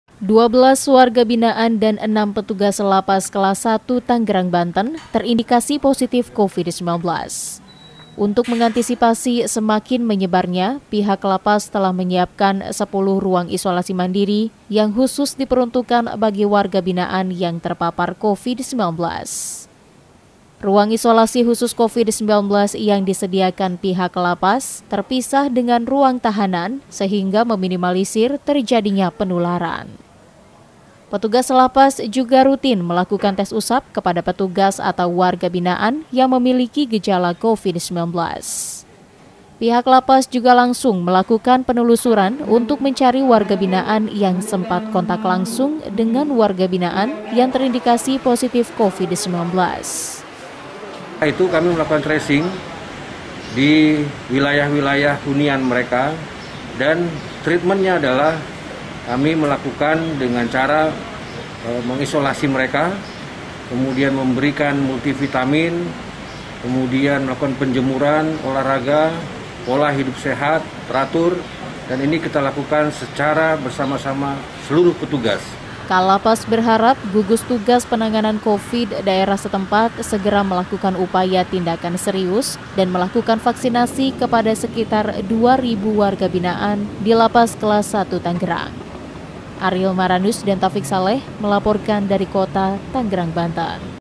BELASAN WARGA BINAAN DAN PETUGAS LAPAS TANGERANG POSITIF COVID-19
UNTUK MENGHINDARI PENYEBARAN COVID-19, LEMBAGA PEMASYARAKATAN KELAS SATU TANGERANG, BANTEN, MENYIAPKAN RUANG ISOLASI KHUSUS COVID-19, PASCA BELASAN NARAPIDANA TERINDIKASI TERPAPAR VIRUS COVID-19.

SELAIN ITU PETUGAS JUGA MEMINTA GUGUS TUGAS PENANGANAN COVID 19 DAERAH SETEMPAT SECEPATNYA TURUN TANGAN DAN MEMBERIKAN VAKSINASI KEPADA RIBUAN WARGA BINAAN.